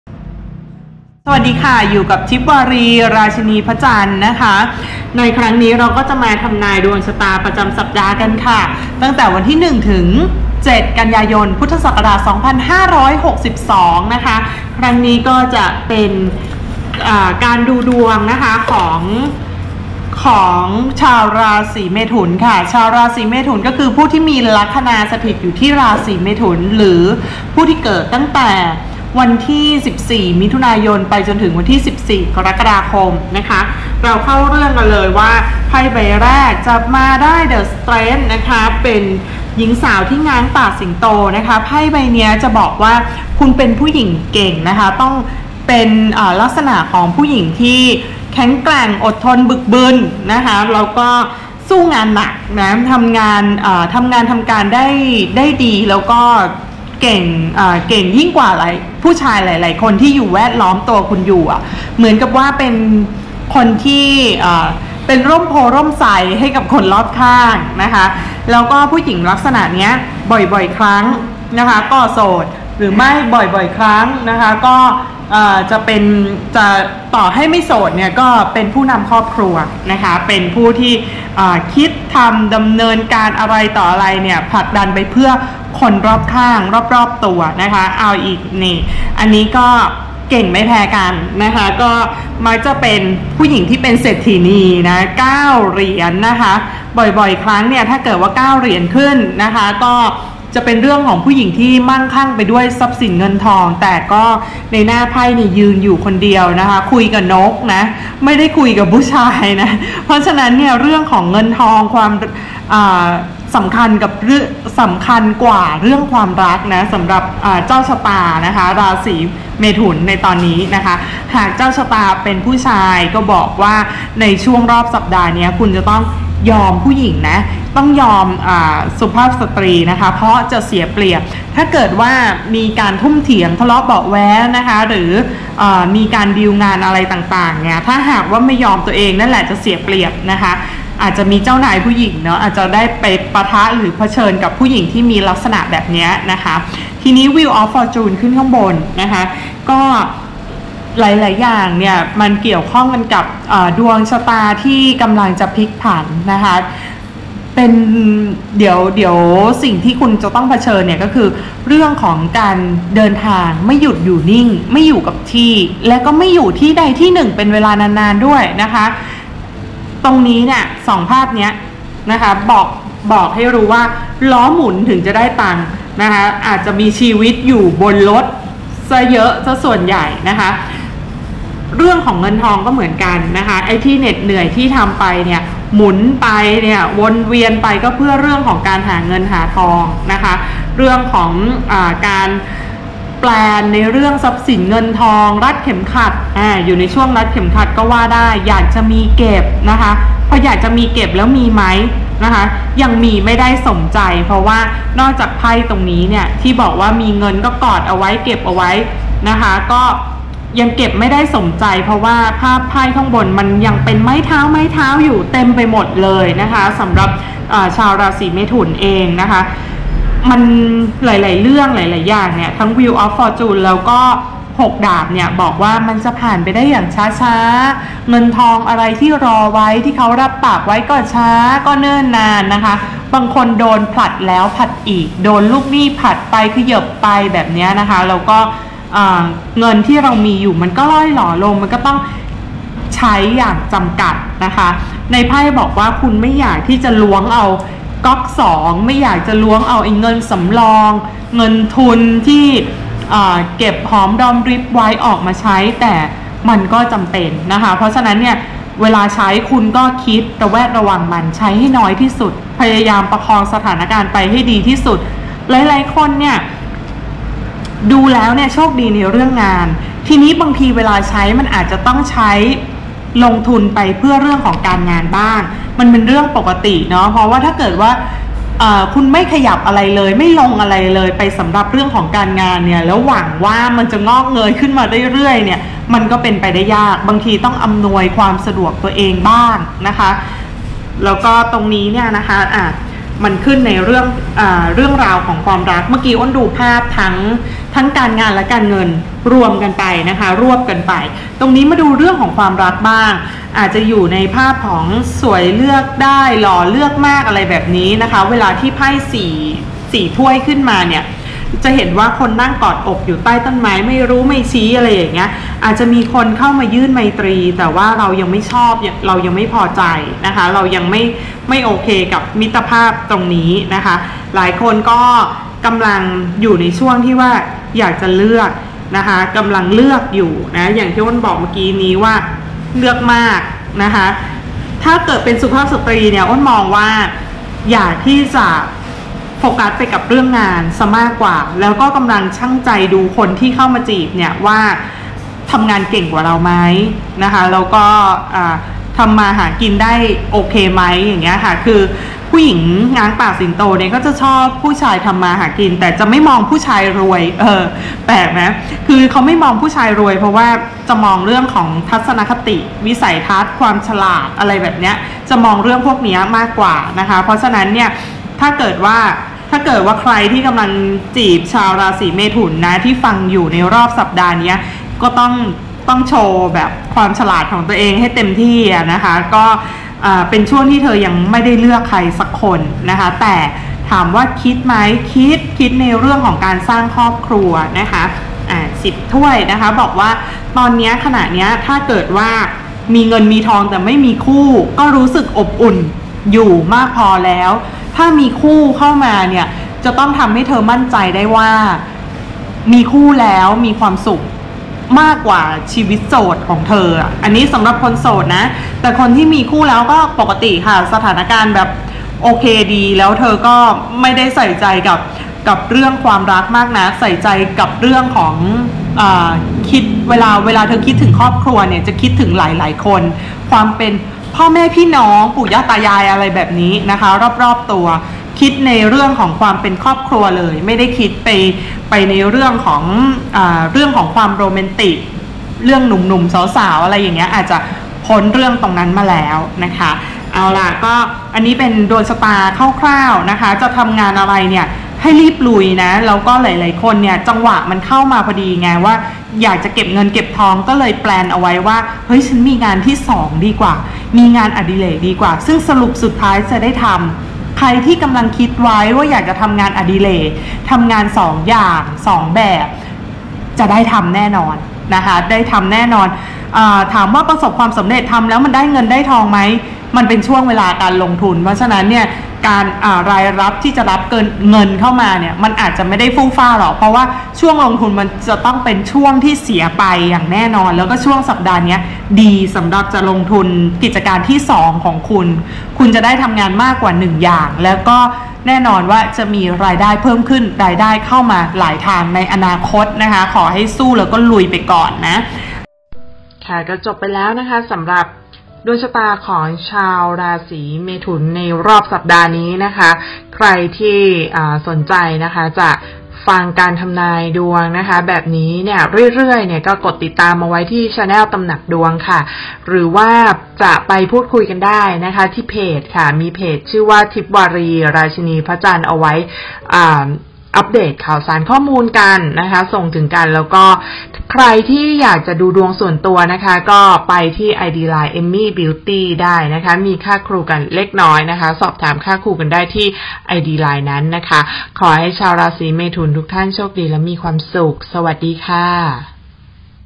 ราศีมิถุน แข็งแกร่งเป็นที่พึ่งให้คนอื่น คิดแต่เรื่องงานจนลืมความรัก
ดูดวงประจำสัปดาห์ ระหว่างวันที่ 1-7 กย. 62  / สำหรับราศีพฤษภ ผู้ที่เกิดระหว่างวันที่ 14 มิย.-14 กค.
มีพลังเหลือเฟือที่จะผลักดันงานไปให้ลุล่วง เป็นช่วงเวลาเหนื่อยเองรวยเอง หากคุณเป็นเพศหญิงดวงแข็งแกร่งมาก ยืนด้วยลำแข้งตนเองและยังเป็นที่พึ่งให้คนรอบข้าง เก่งจนไม่ต้องง้อผู้ชายหรือคู่ครอง แต่หากเป็นผู้ชายก็จะได้เจอผู้หญิงลักษณะดังกล่าว โดนลูกหนี้ผัดผ่อนจนเหลืออด และคุณกำลังกังวลเรื่องเงินเก็บที่พร่องลงไป